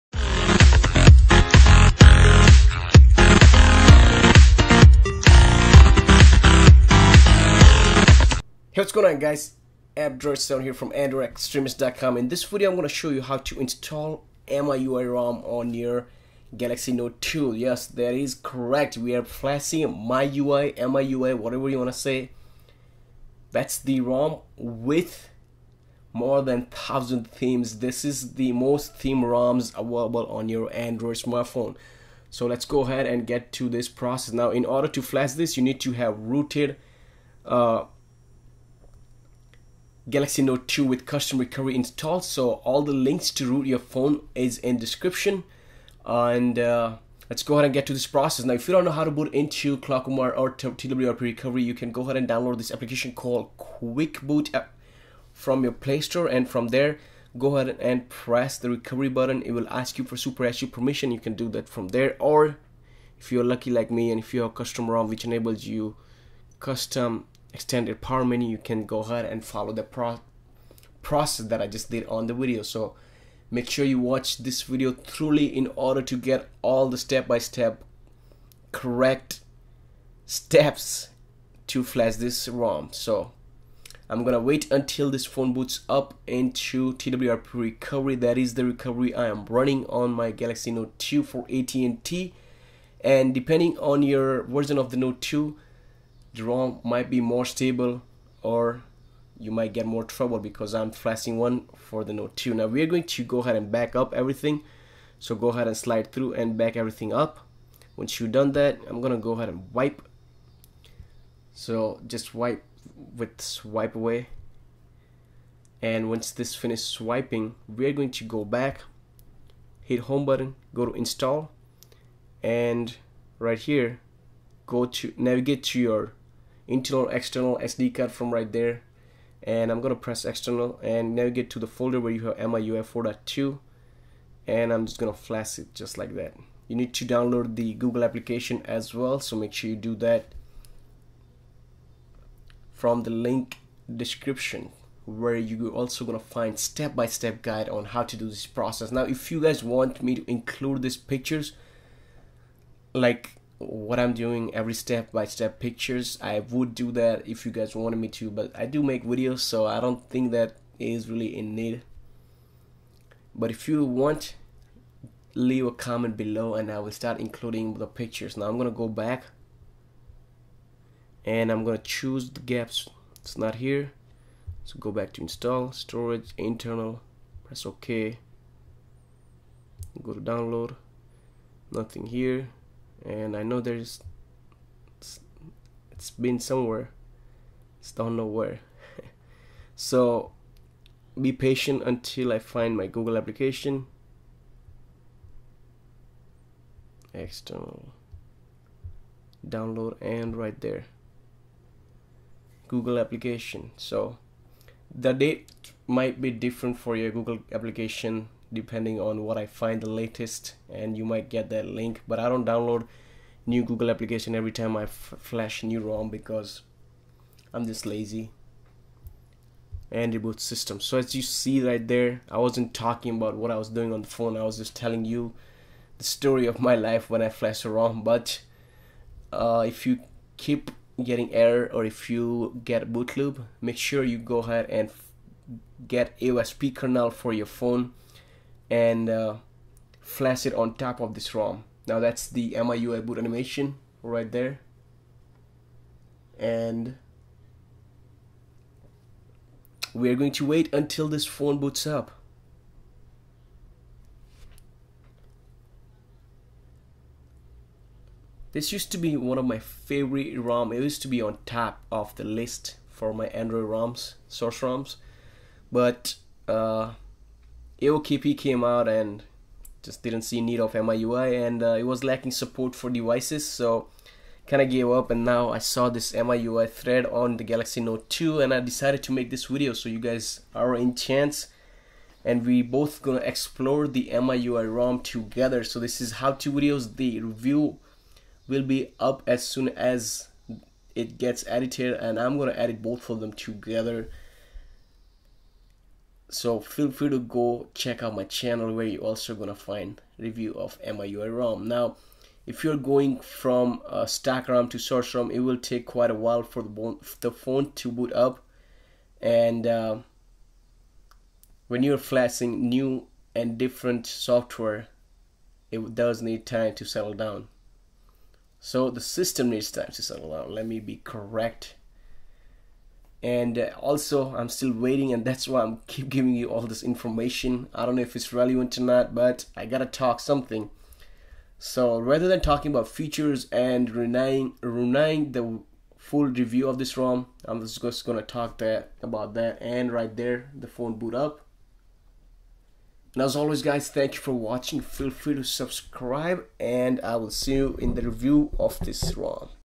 How to Install MIUI Rom on Samsung Galaxy Note 2
This is How to Install MIUI Rom on Samsung Galaxy Note 2.

Here is link to Rom files and brief how to guide.

Root Your Note 2!